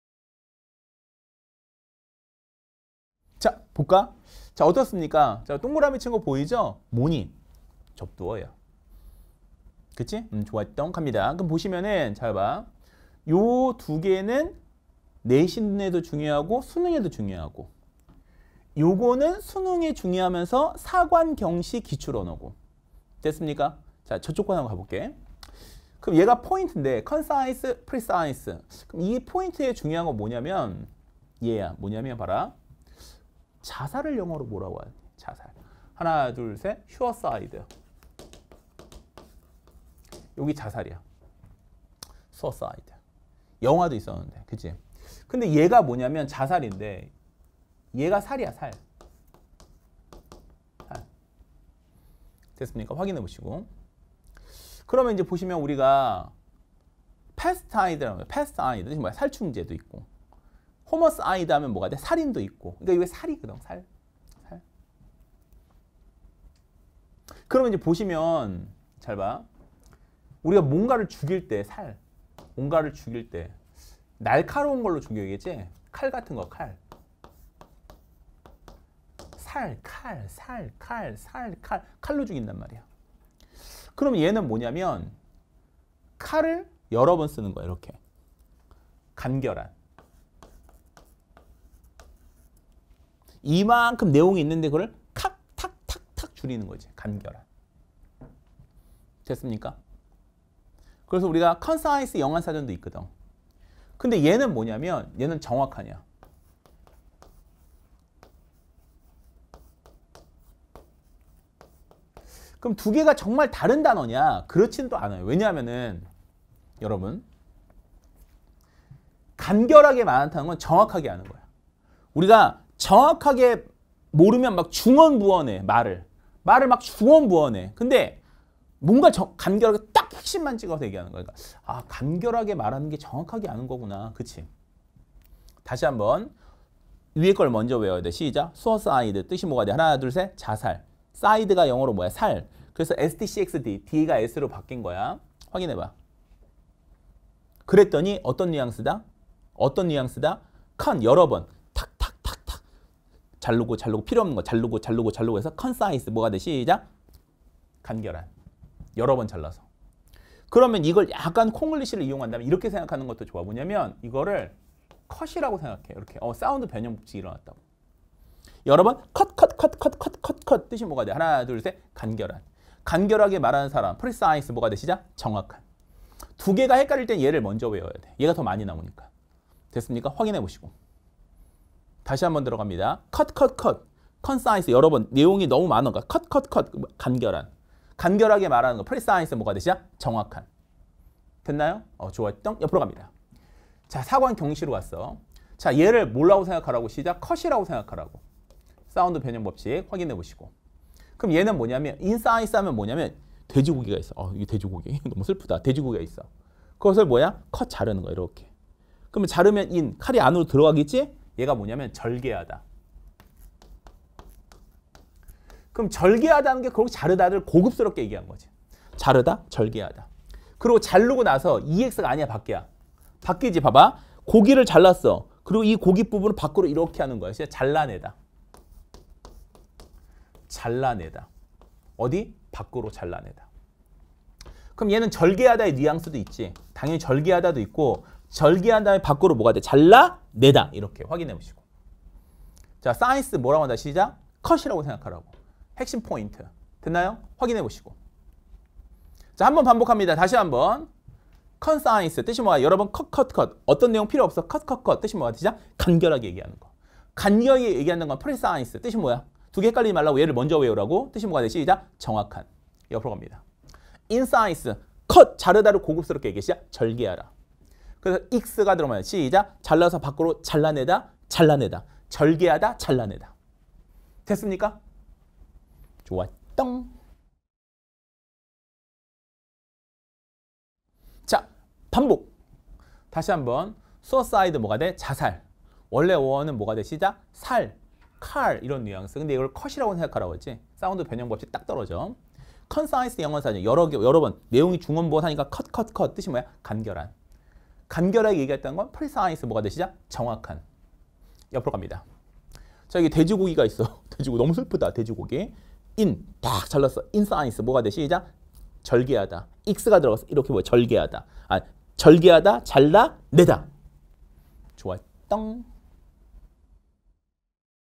[메가스터디] 영어 단어암기 김동영쌤 - 혼동 어휘 특강 (30)
고 1,2 학생이 알아야 할 혼동 어휘 특강입니다.

내신,수능 그리고 사관·경찰 시험에서도 중요한 단어로 시작합니다!

지금 미니 특강을 통해 바로 확인하세요!